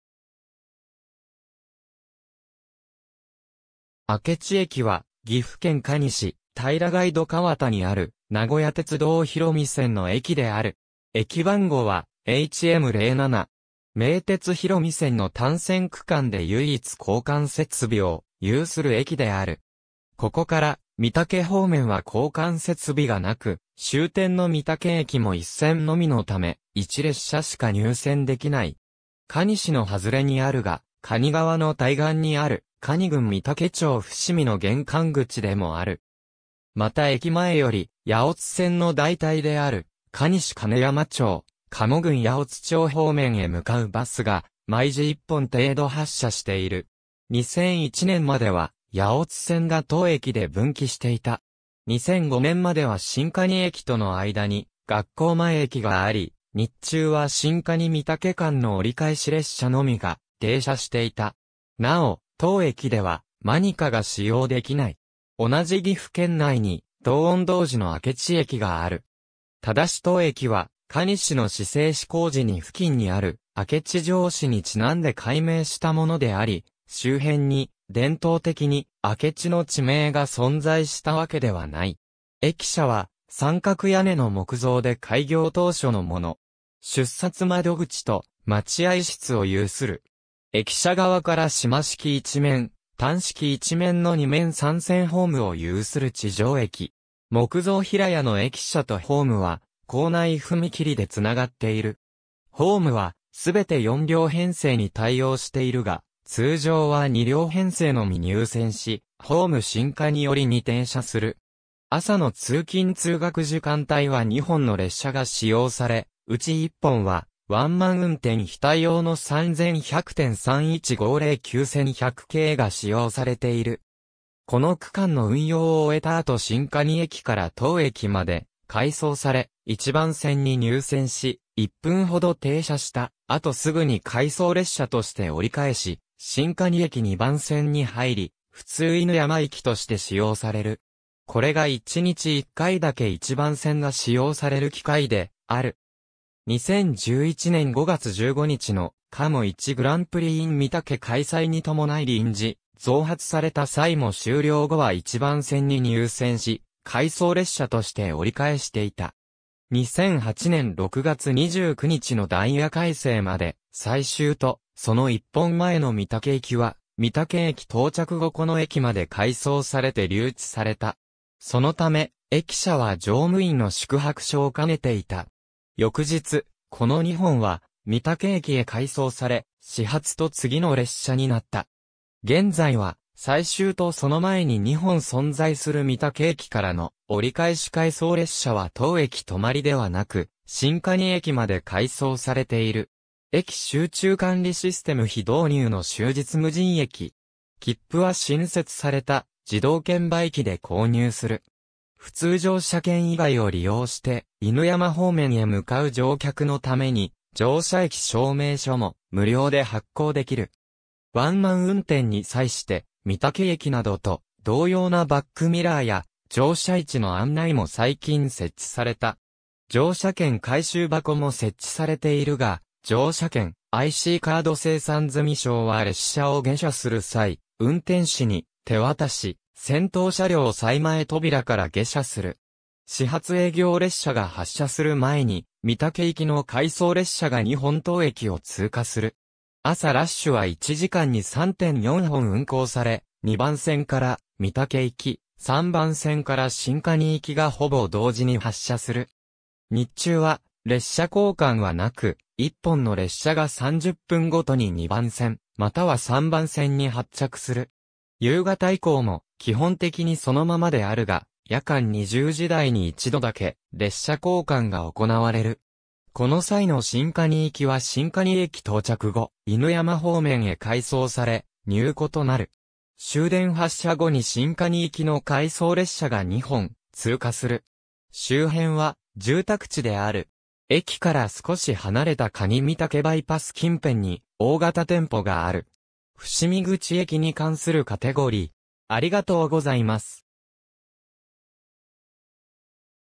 明智駅 (岐阜県可児市)
明智駅（あけちえき）は、岐阜県可児市平貝戸川田にある名古屋鉄道広見線の駅である。
駅番号はHM07。
名鉄広見線の単線区間で唯一交換設備を有する駅である。
ここから御嵩方面は交換設備がなく、終点の御嵩駅も1線のみのため、1列車しか入線できない。
可児市の外れにあるが、可児川の対岸にある可児郡御嵩町伏見の玄関口でもある。
また駅前より八百津線の代替である可児市兼山町・加茂郡八百津町方面へ向かうバス（YAOバス）が毎時1本程度発車している。
2001年（平成13年）までは八百津線が当駅で分岐していた。
2005年（平成17年）までは新可児駅との間に学校前駅があり、日中は新可児 - 御嵩間の折り返し列車のみが停車していた。
なお、当駅ではmanacaが使用できない。
同じ岐阜県内に同音同字の明智駅（明知鉄道、恵那市・旧恵那郡明智町）がある。
ただし当駅は可児市の市制施行時に付近にある明智城址にちなんで改名したものであり、周辺に伝統的に明智の地名が存在したわけではない。
駅舎は三角屋根の木造で開業当初のもの。
出札窓口と待合室を有する。
駅舎側から島式1面（1・2番線）、単式1面（3番線）の2面3線ホームを有する地上駅。
木造平屋の駅舎とホームは構内踏切で繋がっている。
ホームは全て4両編成に対応しているが通常は2両編成のみ入線し、ホーム新可児寄りに停車する。
朝の通勤通学時間帯は2本の列車が使用され、うち1本はワンマン運転非対応の3100・3150・9100系が使用されている。
この区間の運用を終えた後新可児駅から当駅まで回送され、1番線に入線し、1分ほど停車した後すぐに回送列車として折り返し、新可児駅2番線に入り、普通犬山行きとして使用される。
これが1日1回だけ1番線が使用される機会である（2番線で折り返す場合もある）。
2011年（平成23年）5月15日の「かも1グランプリin御嵩」開催に伴い臨時増発された際も終了後は1番線に入線し、回送列車として折り返していた。
2008年（平成20年）6月29日のダイヤ改正まで、最終とその1本前の御嵩行きは、御嵩駅到着後この駅まで回送されて留置された。
そのため、駅舎は乗務員の宿泊所を兼ねていた。
翌日、この2本は御嵩駅へ回送され始発と次の列車になった。
現在は最終とその前に2本存在する御嵩駅からの折り返し回送列車は当駅止まりではなく、新可児駅まで回送されている。
駅集中管理システム非導入の終日無人駅。
切符は新設された自動券売機（1台、タッチパネル式で片道普通乗車券のみ購入可能。manaca非対応。故障時などに使うインターホンは犬山駅につながる）で購入する。
普通乗車券以外（manaca･定期券など）を利用して犬山方面へ向かう乗客のために、乗車駅証明書も無料で発行できる。
ワンマン運転に際して、御嵩駅などと同様なバックミラーや乗車位置の案内も最近設置された。
乗車券回収箱も設置されているが、乗車券・ICカード精算済証は列車を下車する際、運転士に手渡し、先頭車両最前扉から下車する。
始発営業列車が発車する前に御嵩行きの回送列車が2本当駅を通過する。
朝ラッシュは1時間に3・4本運行され、2番線から御嵩行き、3番線から新可児行きがほぼ同時に発車する。
日中は列車交換はなく、1本の列車が30分毎に2番線または3番線に発着する。
夕方以降も基本的にそのままであるが、夜間20時台（当駅20:49発）に1度だけ列車交換が行われる。
この際の新可児行きは新可児駅到着後、犬山方面へ回送され、入庫となる。
終電発車後に新可児行きの回送列車が2本通過する。
周辺は住宅地である。
駅から少し離れた可児御嵩バイパス近辺に大型店舗がある。
伏見口駅に関するカテゴリ: